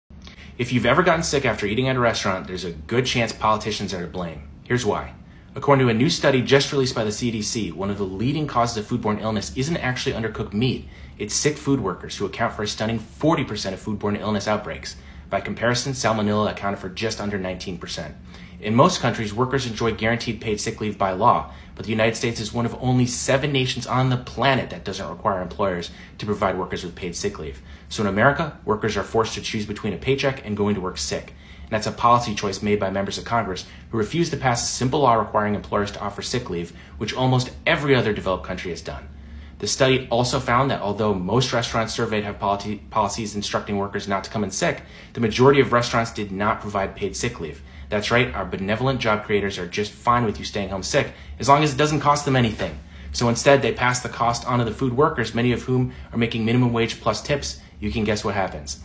New Study Reveals Shocking Cause of Most Food Poisoning
How Washington's inaction puts us all at risk of food-borne illness.